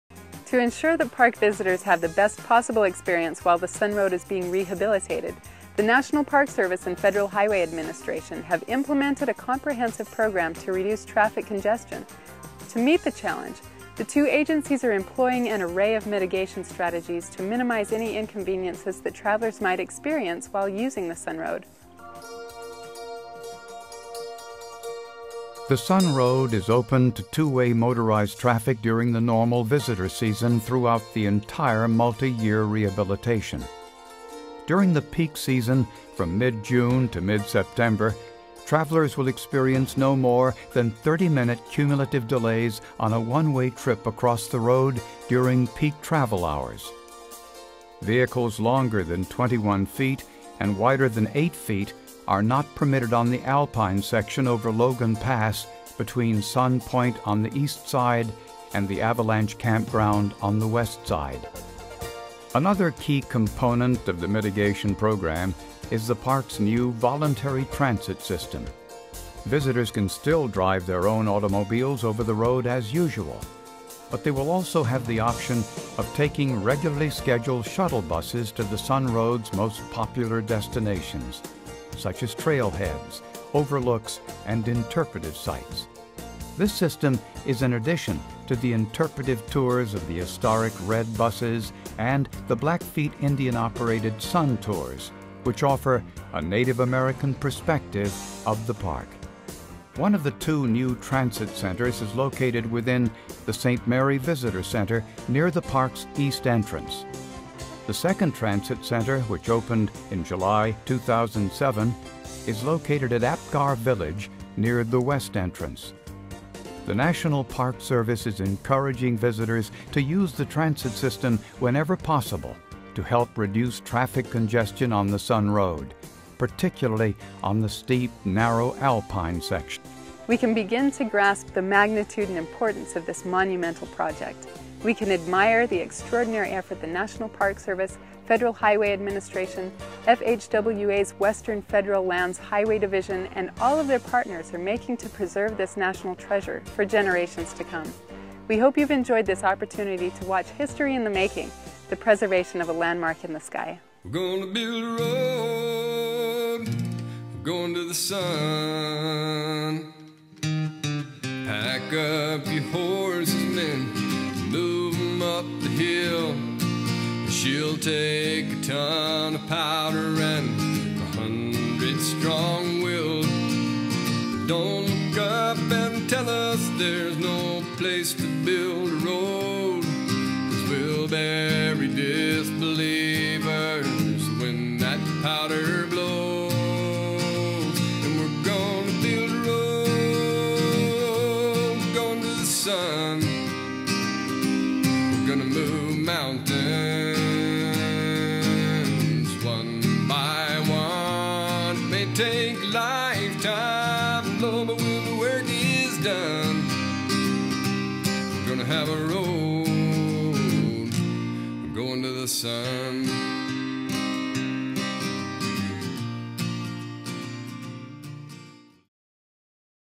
Preserving a Landmark in the Sky: Part 2 of 2
This is the part 2 of 2 of the Preserving a Landmark in the Sky video documenting the reconstruction of Going to the Sun Road in Glacier National Park, Montana.